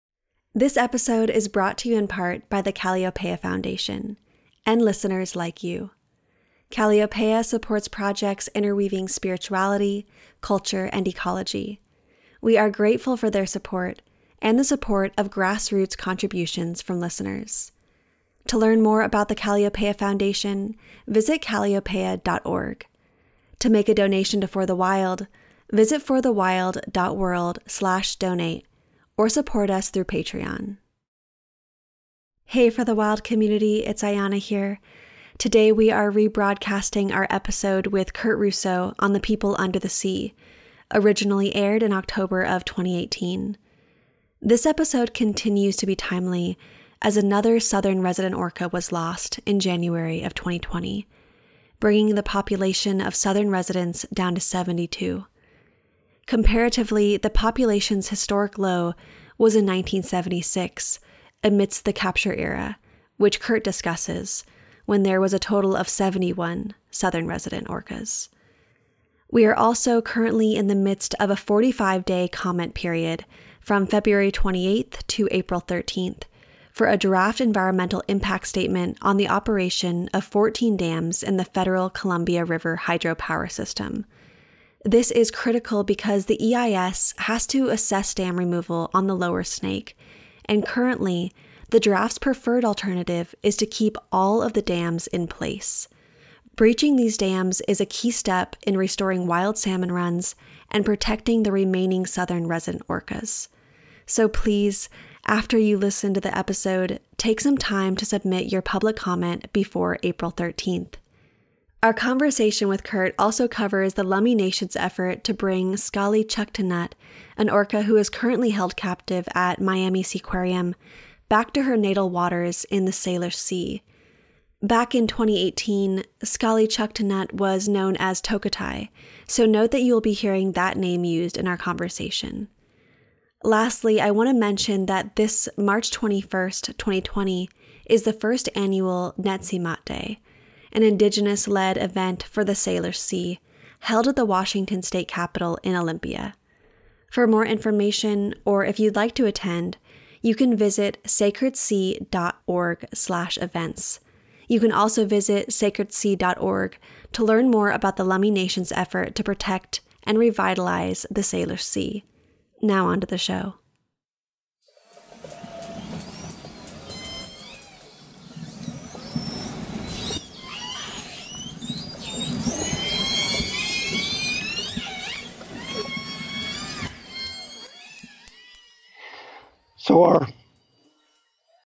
SPECIAL ENCORE - BREACH THE SNAKE RIVER DAMS - KURT RUSSO on the People Under the Sea /162
This week we are rebroadcasting our episode with Kurt Russo on the People Under the Sea, originally aired in October of 2018. Kurt and Ayana’s conversation explores the powerful memory held by Southern Resident orcas, the threats they face from vessel noise, chemical pollutants, and declining Chinook salmon population, the health of the Salish Sea, and the Lummi Nation’s sacred duty to return Sk’aliCh’elh-tenaut (formerly known as Tokitae/Lolita), from where she is being held captive at Miami Seaquarium, to her natal waters in the Salish Sea. This episode remains timely as another Southern Resident orca was presumed dead in late January 2020, bringing the total population of Southern Residents down to 72, the second-lowest it has ever been since official counting began.

Action Points

Call Governor Jay Inslee at and insist that Washington take the appropriate measures, as outlined in the Southern Resident Orca Task Force’s Final Report and Recommendations. It’s important that Gov. Inslee knows that the global community is watching and expects swift action to be taken.

Submit a public comment on the draft environmental impact statement on the operation of 14 dams in the federal Columbia River hydropower system. The comment period is from 2/28/20 to 4/13/20. This is critical because the EIS has to assess dam removal on the Lower Snake, and currently, the draft’s preferred alternative is to keep all of these dams in place. Breaching these dams is a key step in restoring wild salmon runs and protecting the remaining Southern Resident orcas. It’s important that you write an original comment, for ideas of what to say visit PNW Protectors and March For The Dams. You can submit your comment here.

Write to Miami Seaquarium (4400 Rickenbacker Causeway, Miami, FL 33149) or call at and urge Andrew Hertz, President and General Manager of the Seaquarium, to release and repatriate Sk’aliCh’elh-tenaut (Tokitae/Lolita) to her native waters.

If you are local to the Salish Sea area, attend the first annual Netse Mot on 3/21/20, an Indigenous-led event for the Salish Sea, held at the Washington State Capital in Olympia.

Consider joining the first March For The Dams, a 22-day walk from Portland to the Ice Harbour Dam. The march started 3/1/20 and will end on 3/22/20, it seeks to raise awareness and put pressure on the Army Corps of Engineers, Bonneville Power Administration, and the US government to breach the lower 4 Snake River dams. You can join any day, to learn how to participate, visit March For The Dams.

If you’re local to the Salish Sea area, considering joining Protectors of the Salish Sea.

Donate directly to Sacred Sea to help efforts to bring Sk’aliCh’elh-tenaut (Tokitae) home.